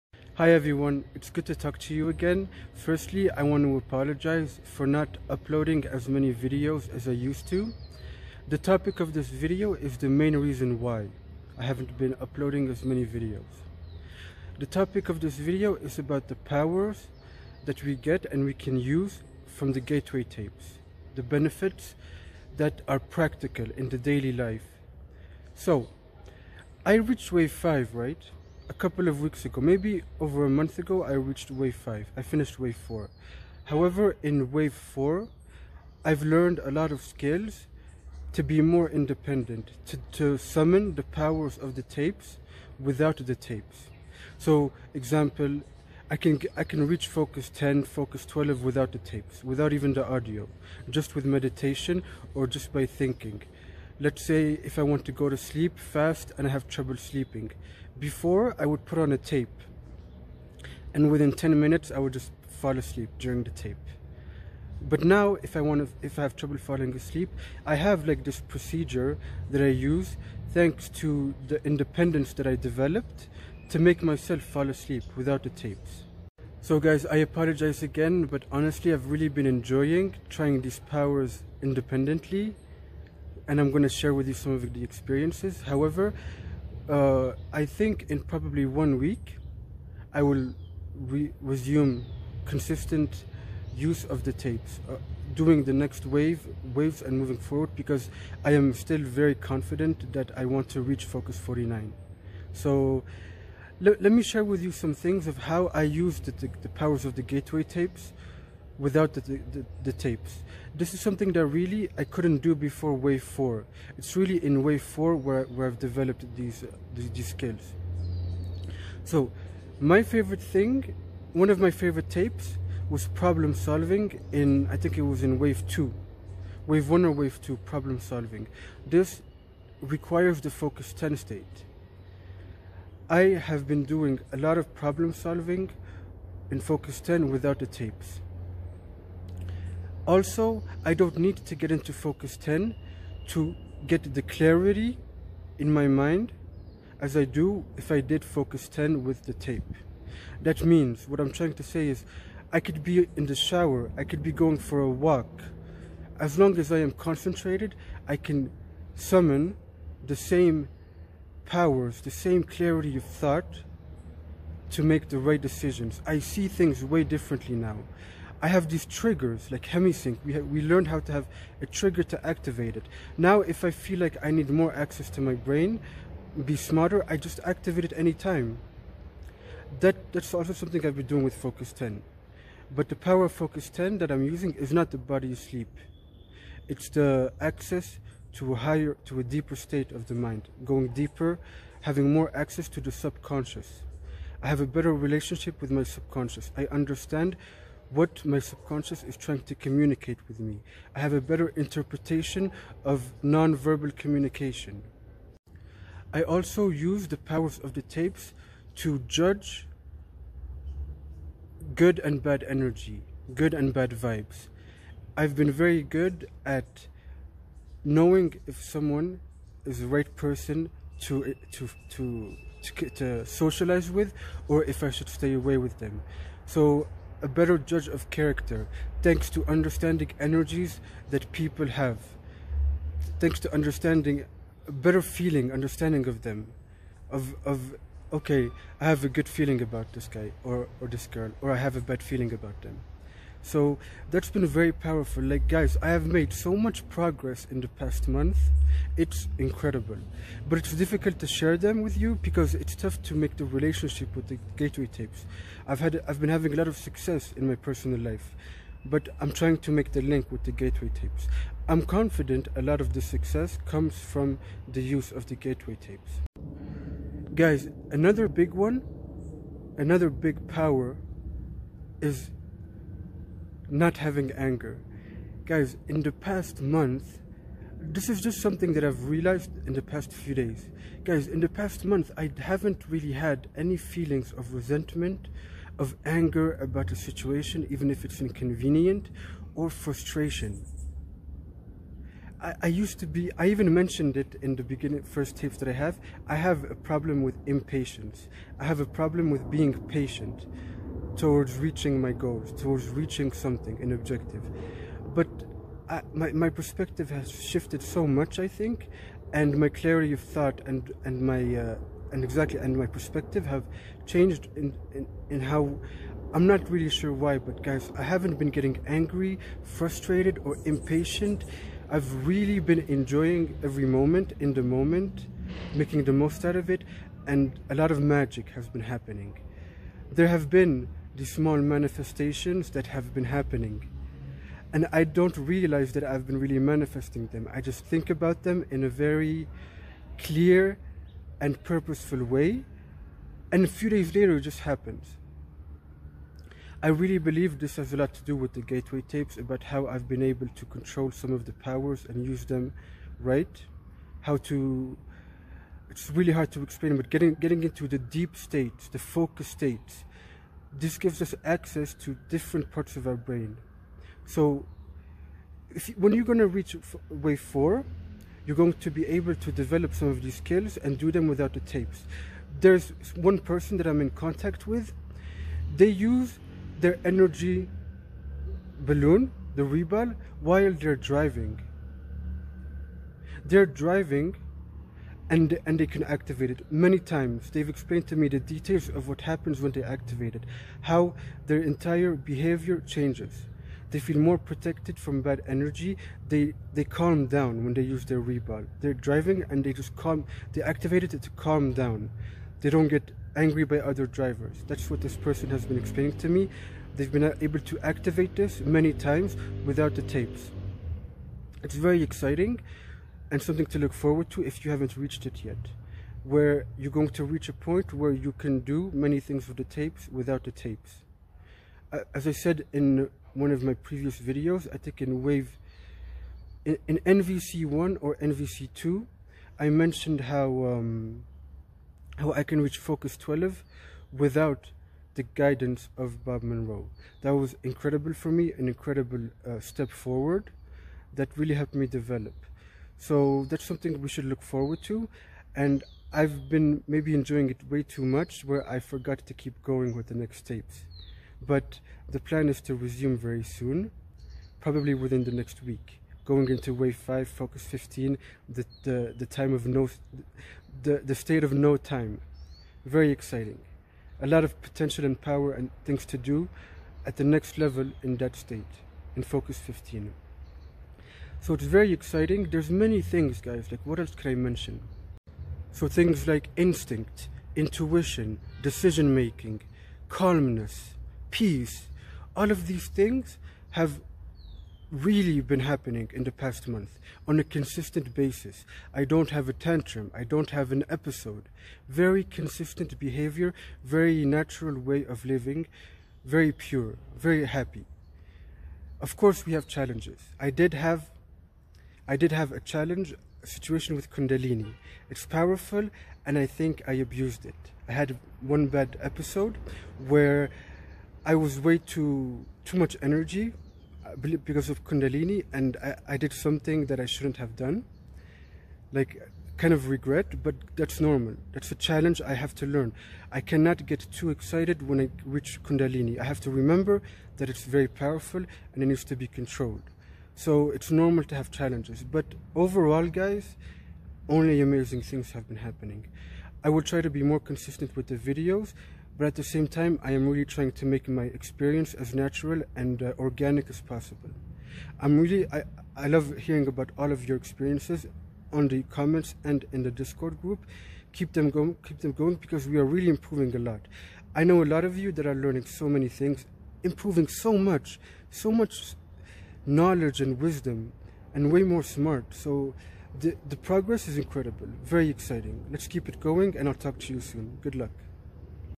Practical Ways to Use the Powers of the Tapes in your Daily Life
Hey everyone!! In this video I wanted to share with you the skills I developed from the Gateway tapes, and how I use them in my daily life.
Some of these benefits include easy sleeping, reaching deep levels of concentration and focus, less anger, more happiness, and more wisdom.